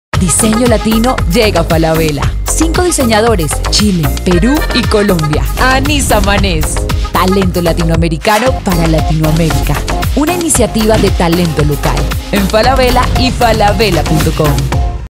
Diseño Latino: Anis Samanez
Prendas con personalidad y con un propósito demostrar el color de Latinoamérica, inspiradas en la cultura Shipibo-conibo. Disponible en tiendas Falabella y Falabella.com